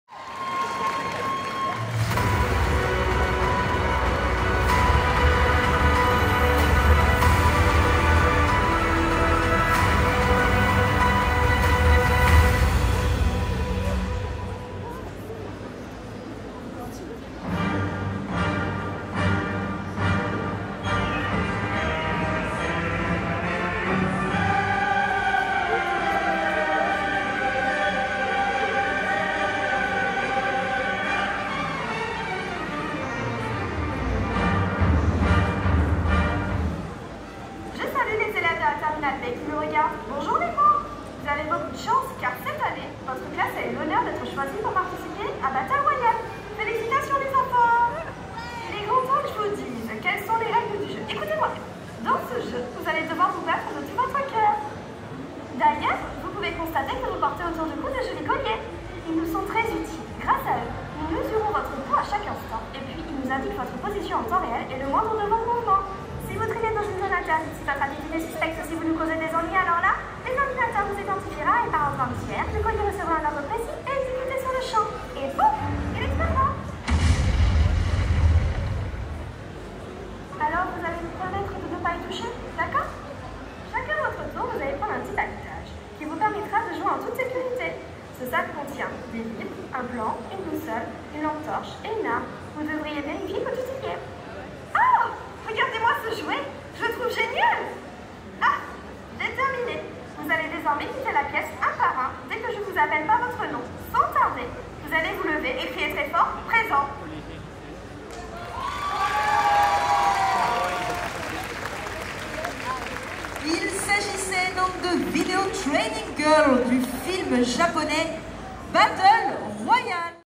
Hero Festival Marseille 2019 - Cosplay contest (Battle Royale)
Hero Festival Marseille 2019  (Marseille// France), Cosplay contest and CFC preliminaries !

Video Training girl
Battle Royale